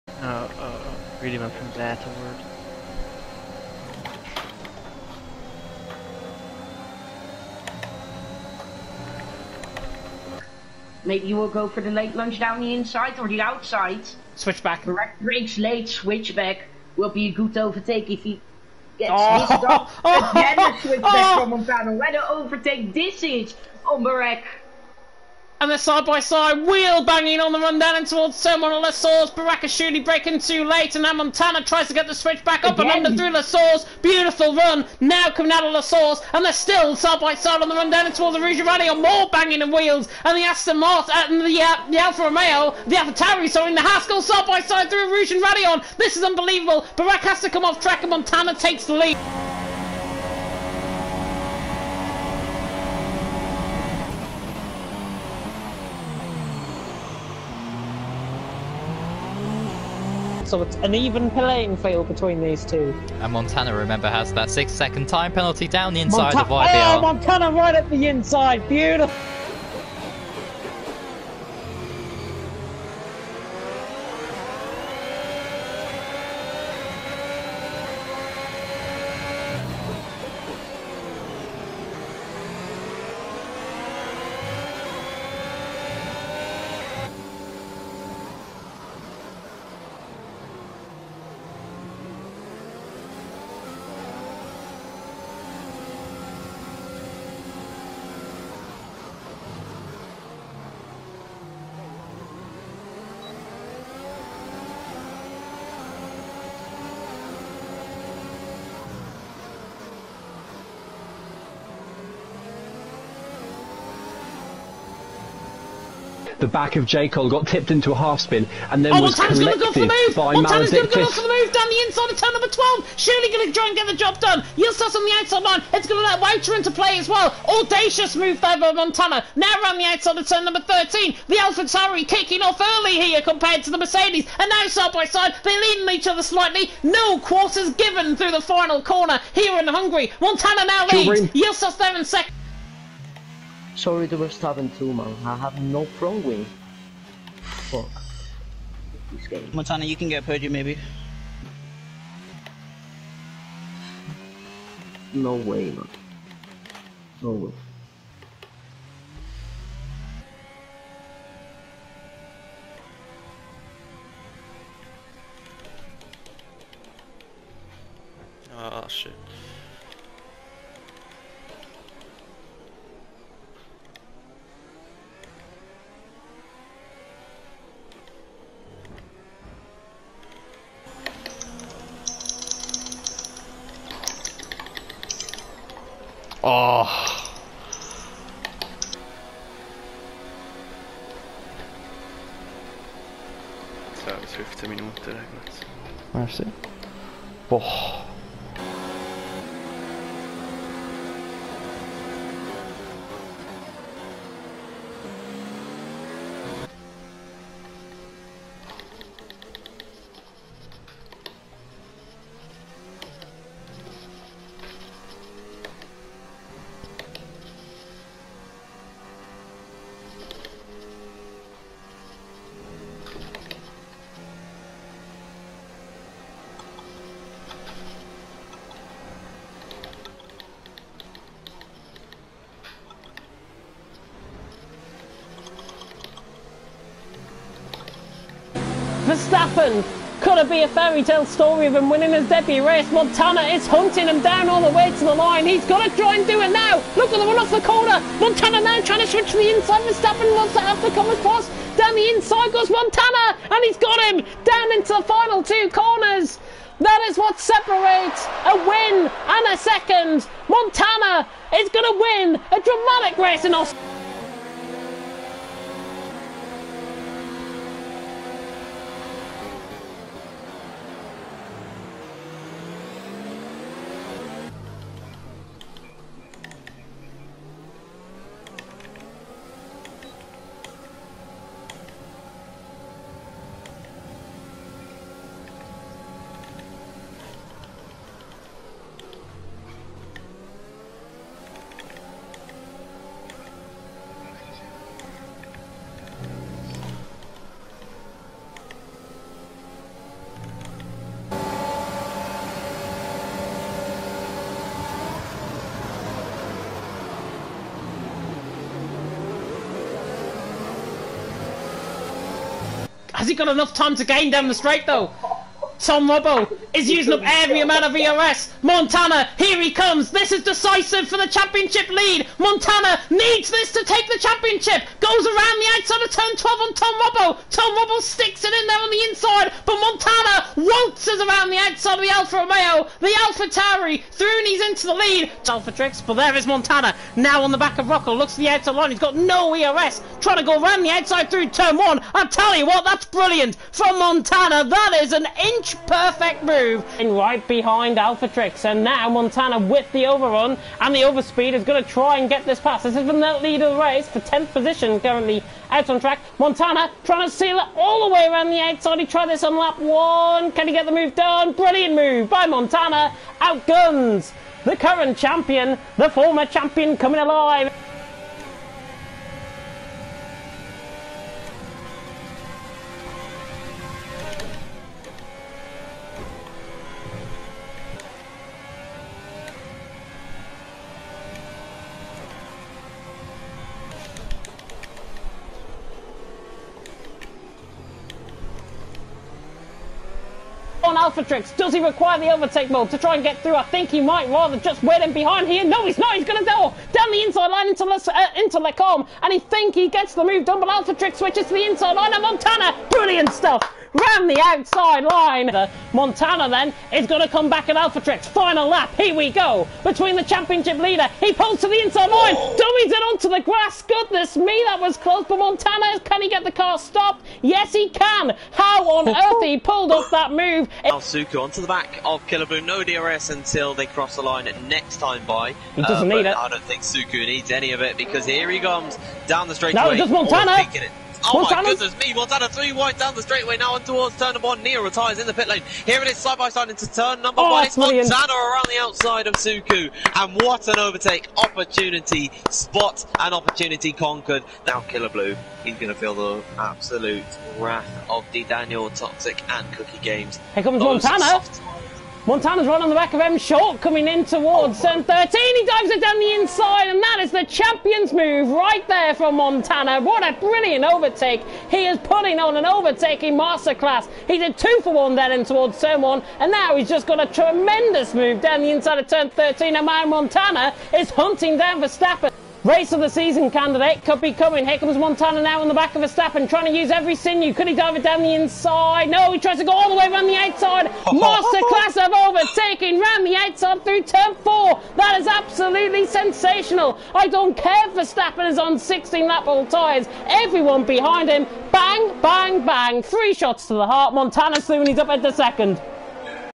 My best overtakes in league racing!
A little collection of my favourite moves over the years...